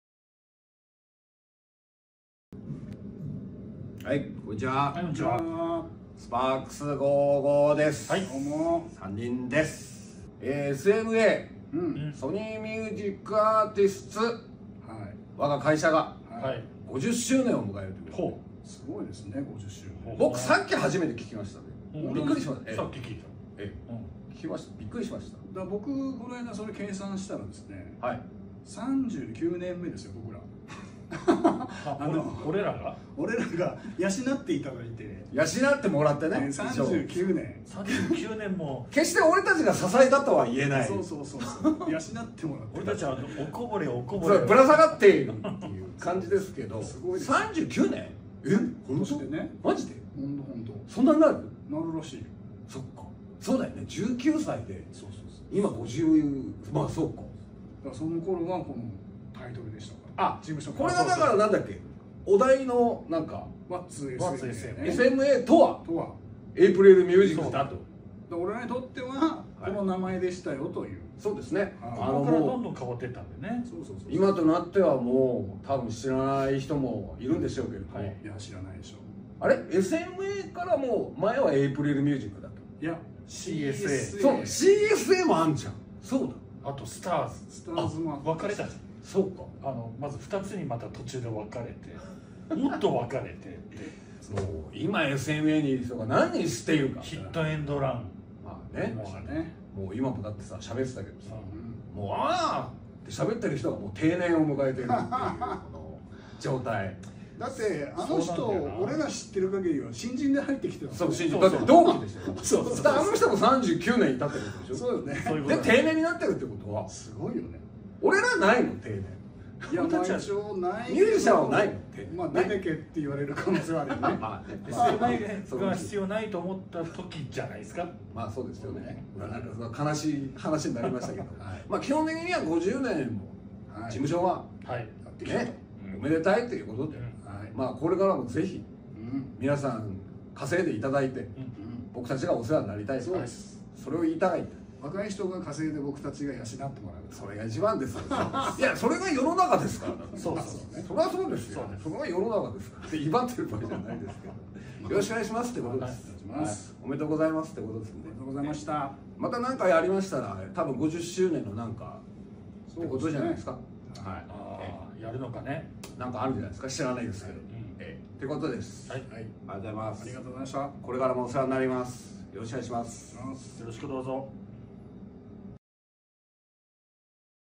📢WHAT'S SMA？📢
今回は
が登場❗️

「あなたにとってのSMAとは？」を
リレー形式でお答えします。